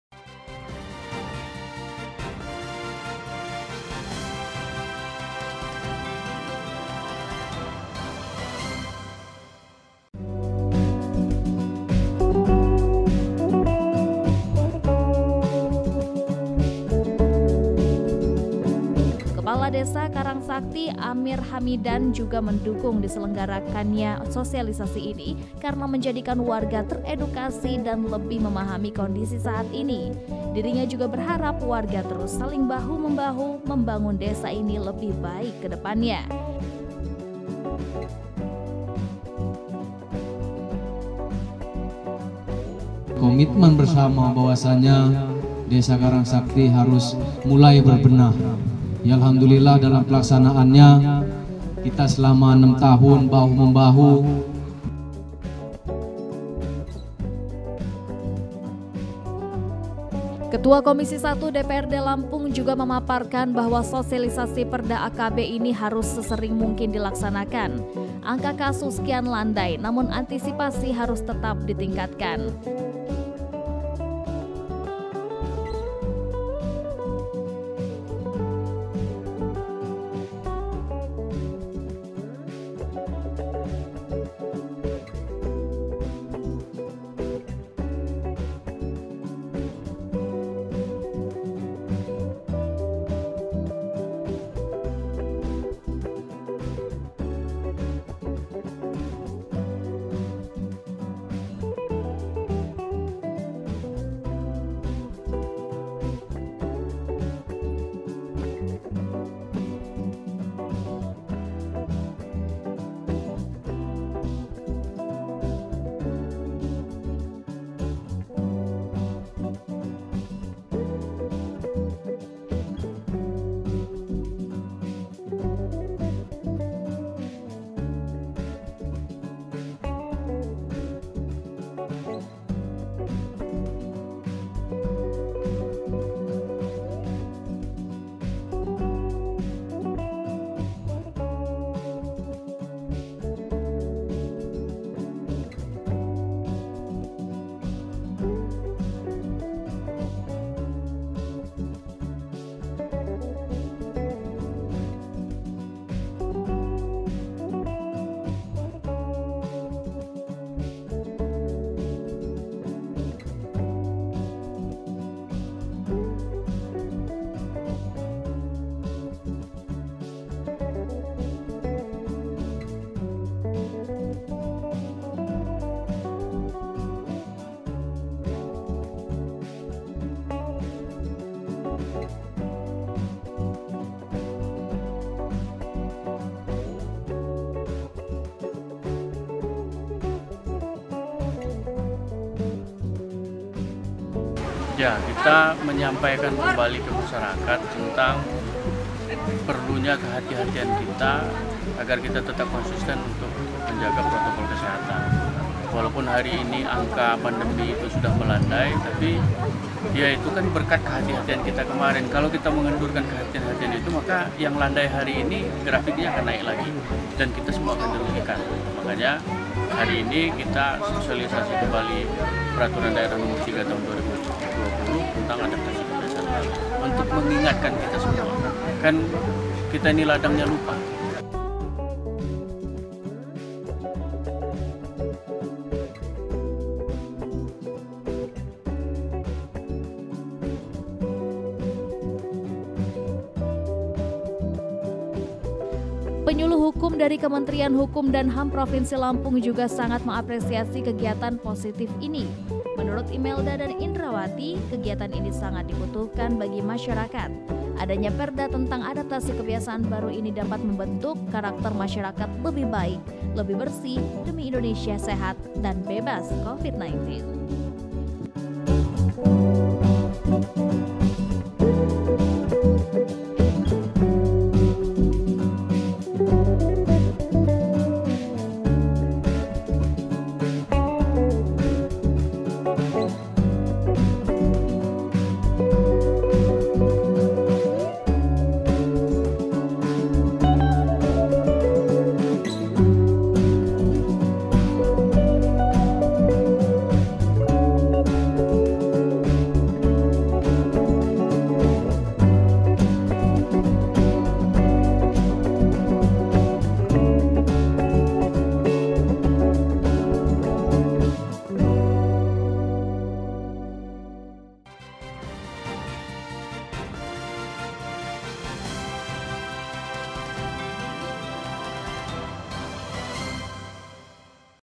Program Spesial - Warga Desa Karang Sakti Antusias Terima Sosialisasi Perda AKB Part 3
Ketua Komisi I DPRD Lampung, Yozi Rizal kembali mensosialisasikan peraturan daerah nomor 3 tahun 2020, tentang adaptasi kebiasaan baru dalam pencegahan dan pengendalian Covid-19. Sosialisasi berlangsung di desa Karang Sakti, kecamatan Muara Sungkai, kabupaten Lampung Utara. Sosialisasi peraturan daerah tentang adaptasi kebiasaan baru, terus disosialisasikan untuk selalu mengingatkan masyarakat tentang pentingnya protokol kesehatan Covid-19 dan juga pentingnya vaksinasi.